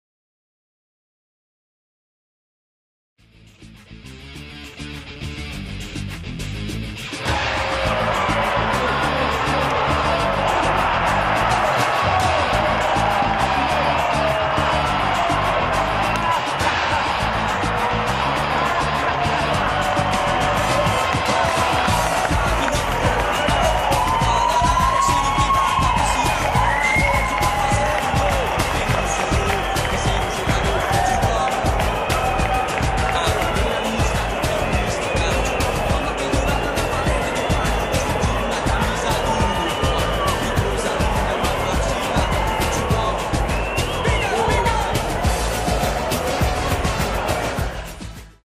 Le slalom victorieux d'un streaker à l'entraînement des Bleus !
Scène cocasse ce vendredi à l'entraînement des Bleus, ouvert au public de Ribeirão Preto, pour la troisième fois depuis le début de l'aventure brésilienne. Déroulé sous un beau soleil et devant plusieurs milliers de personnes, l'entraînement de près de deux heures s'est terminé par l'entrée d'un jeune streaker, qui contrairement à ses compères d'il y a deux semaines, a lui réussi à se jouer de la sécurité pour enlacer un joueur de l'équipe de France, Eliaquim Mangala en l'occurrence. Rattrapé ensuite par la patrouille, le jeune homme, mineur, a été raccompagné au poste de la police militaire situé dans l'enceinte du stade Santa Cruz de Botafogo, sans violence.